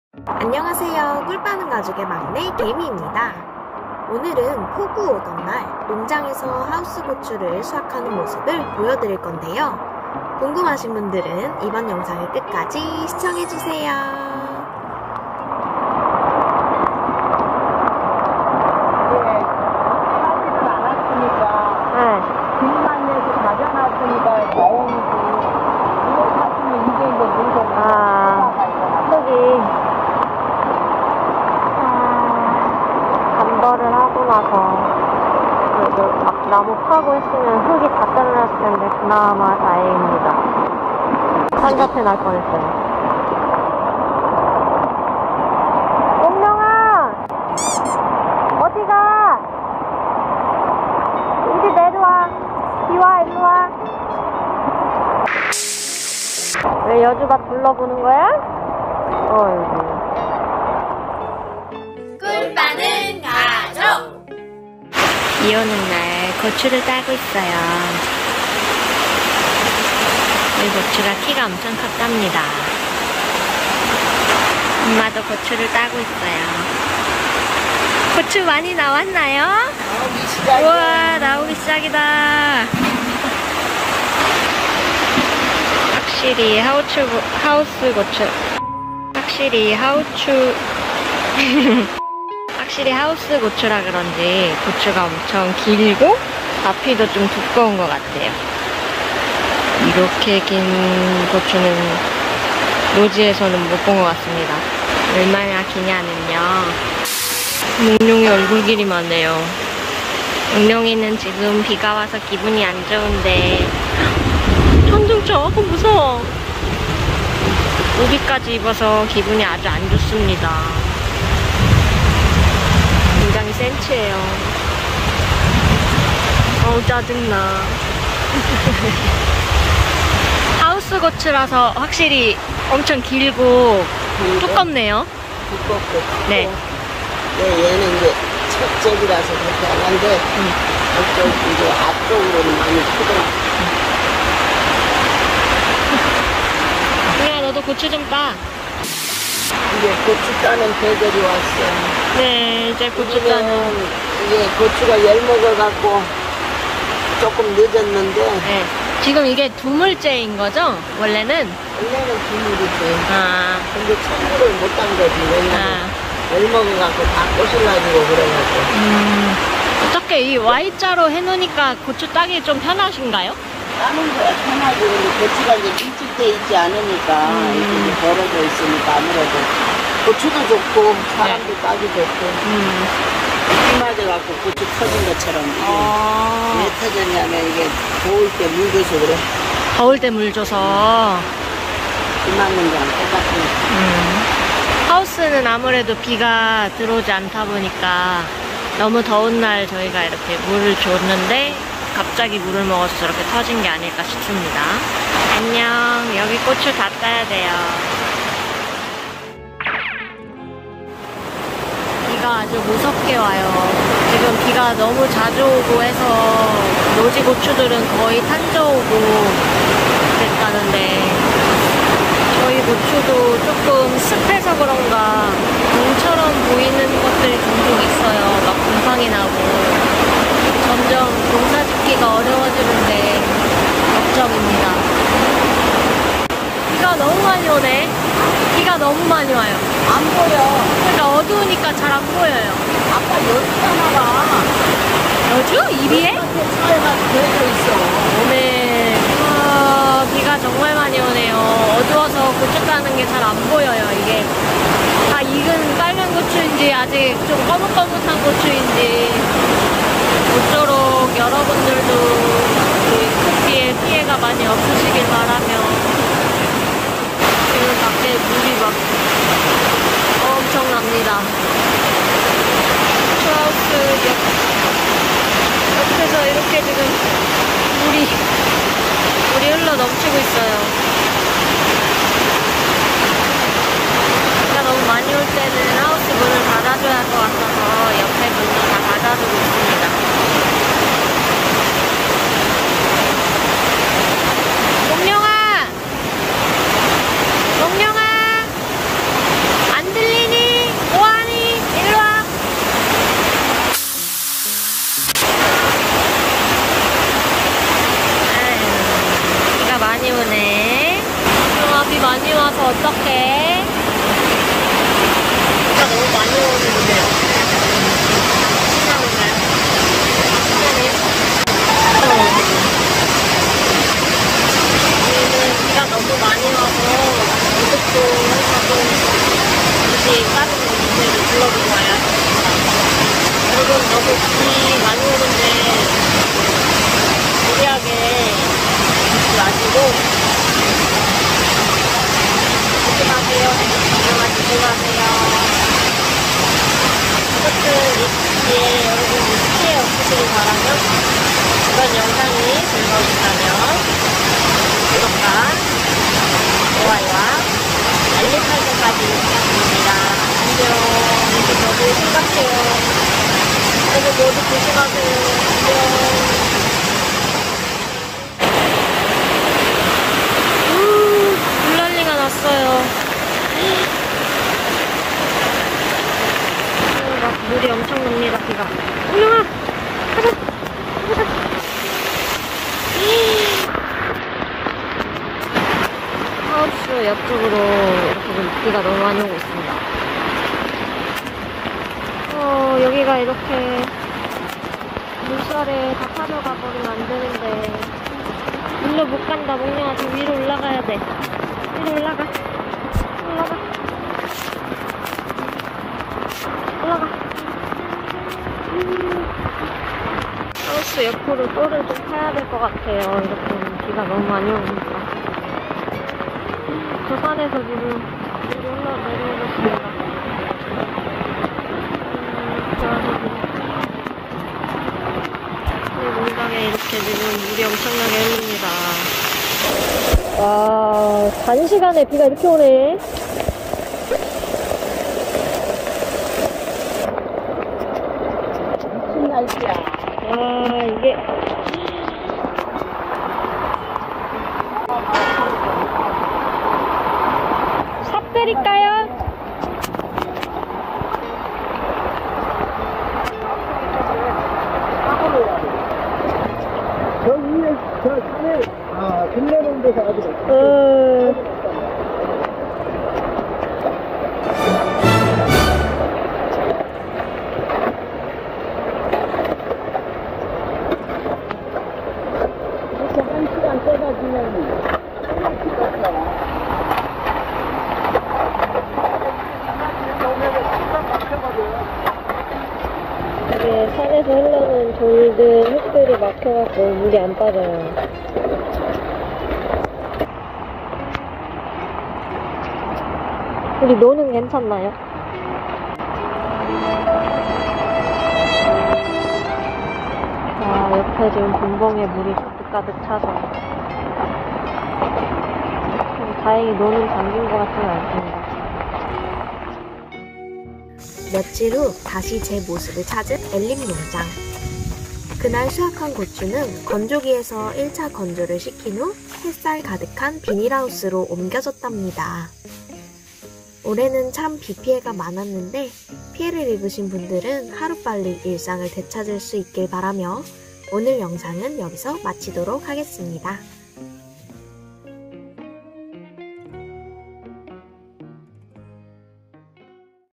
고추 수확! 태풍 영향으로 폭우 오던 날 엘림농장에서 하우스 고추를 수확해요
오늘은 폭우 오던 날 하우스 고추 수확하는 모습을 보여드릴 건데요! 우리 구독자님들 모두 태풍 피해는 없으셨는지 궁금하네요ㅜㅜ 올해는 비도 많이 내리고 태풍도 크게 지나가서 걱정이었는데 모두들 하루 빨리 일상을 되찾을 수 있길 바랍니다!